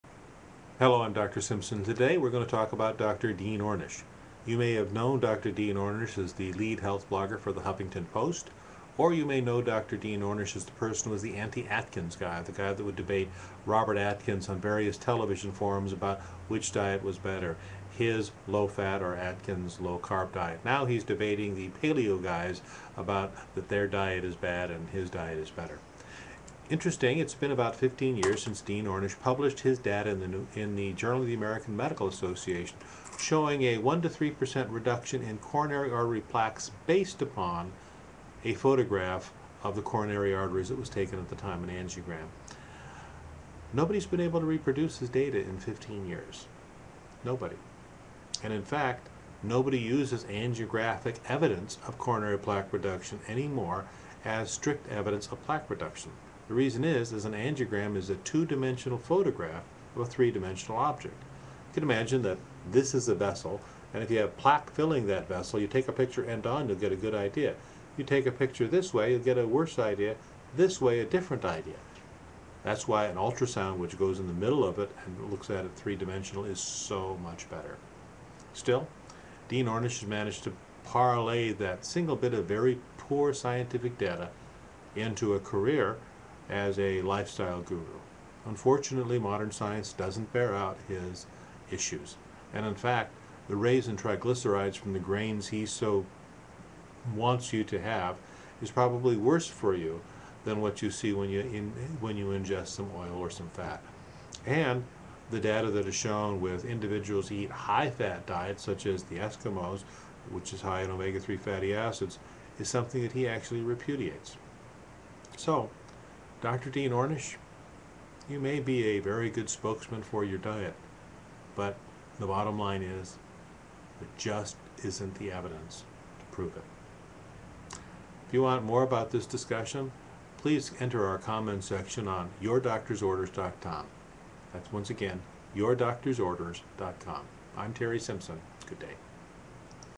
Dr. Terry Simpson on Dr. Dean Ornish
- but the low-fat diet proposed by Dean Ornish simply is not proven in any but his own studies.